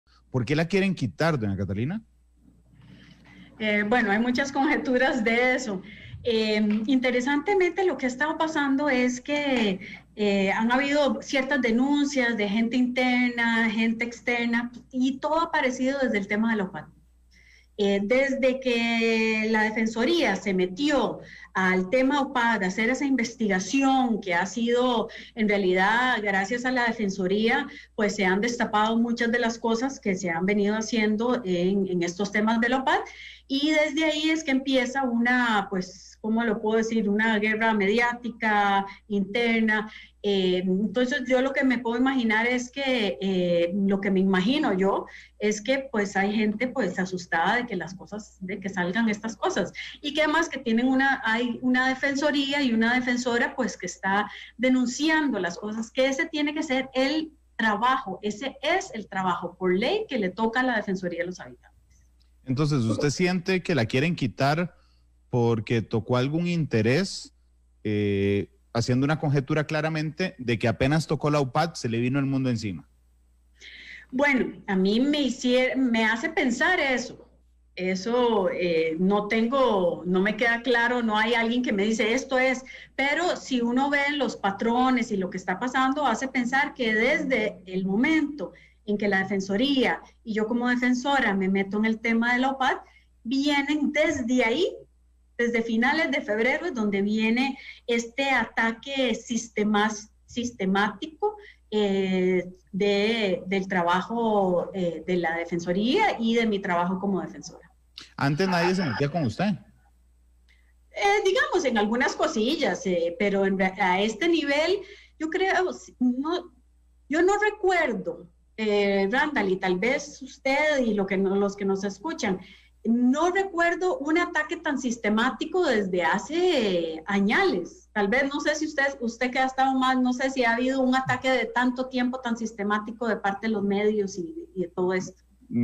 ¿Por qué la quieren quitar doña Catalina?
Esta fue la pregunta que le hice a la defensora de los Habitantes, Catalina Crespo en medio de una especie de juicio político que le lleva adelante la Asamblea Legislativa.